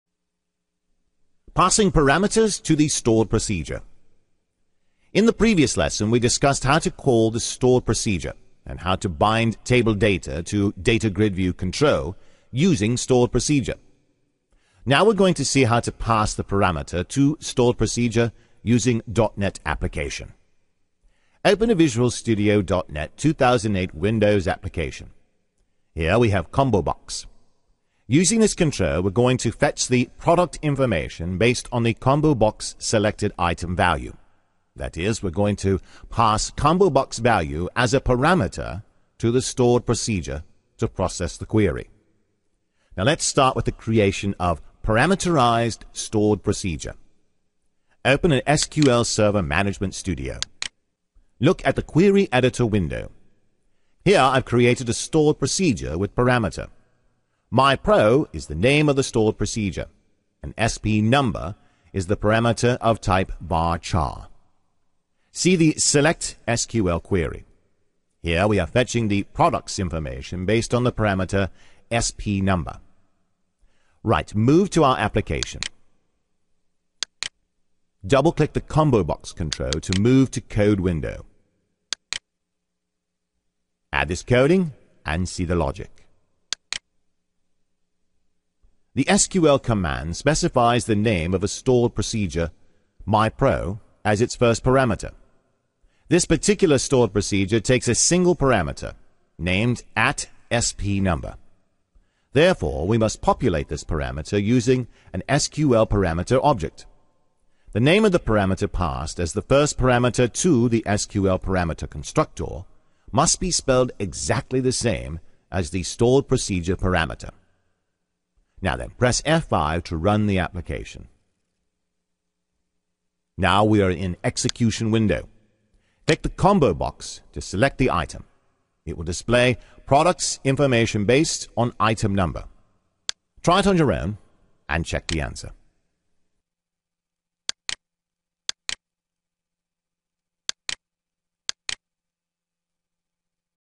Stored procedure - Passing Parameters - Wingslive
This video describes how to pass parameters to stored procedure in C#.NET 2008.